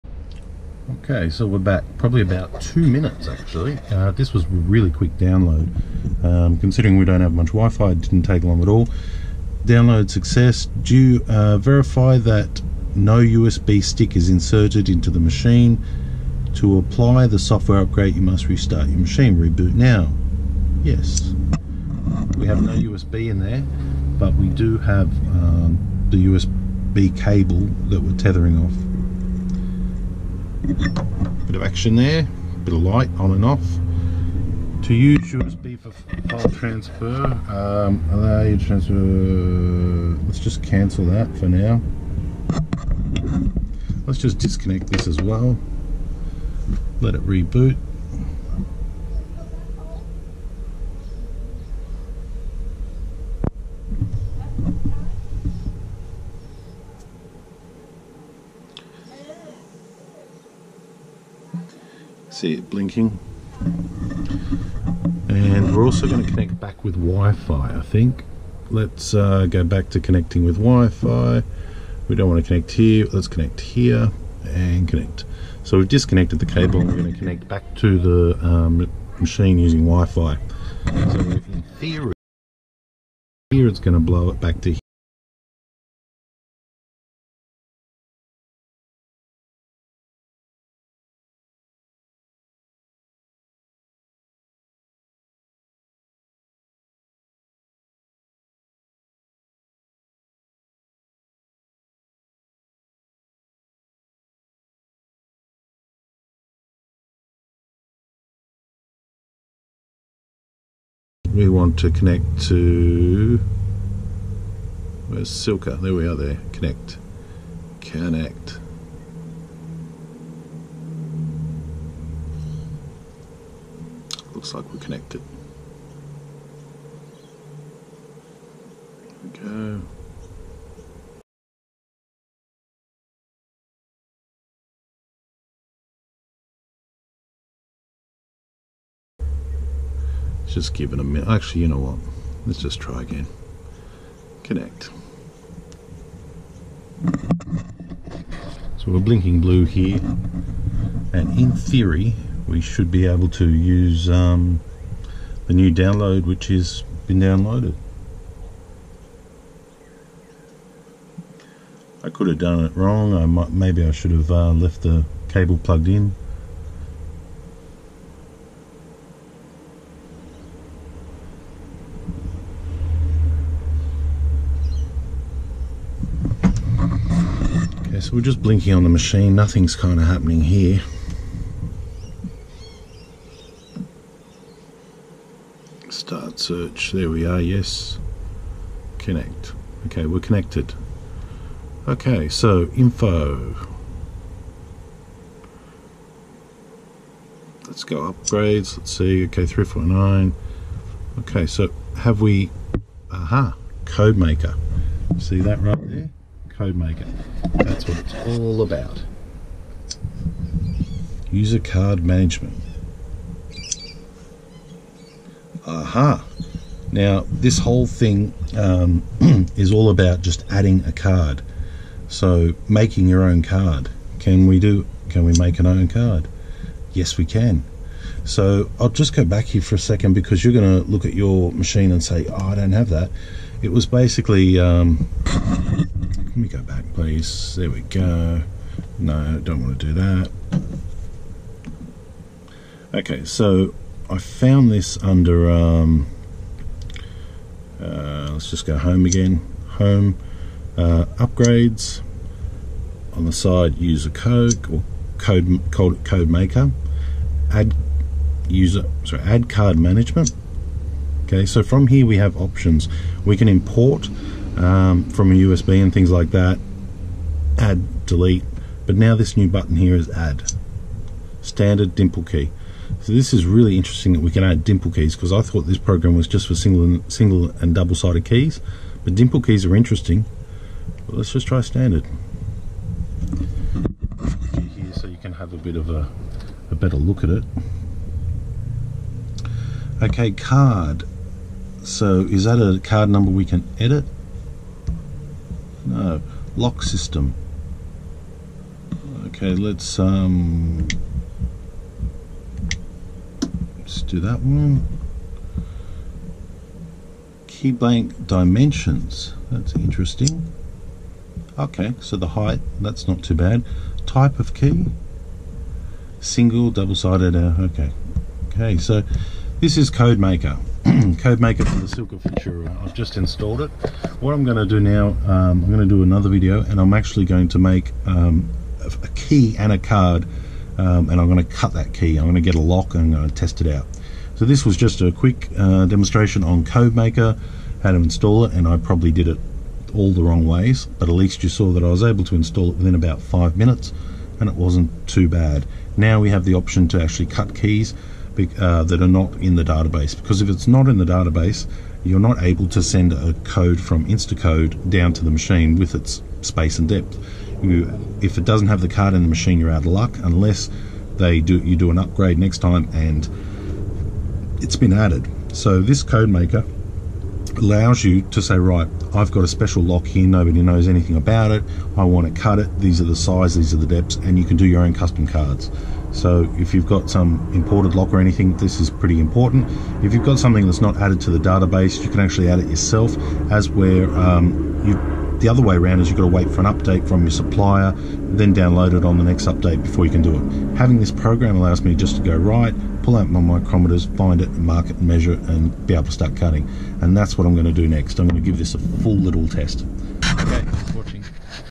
Install Codemaker Silca Futura Pro
Ok so we paid for the update, it was quick to install and now we have code maker on the Silca Futura Pro. This is all about installing the Code maker it's one word = codemaker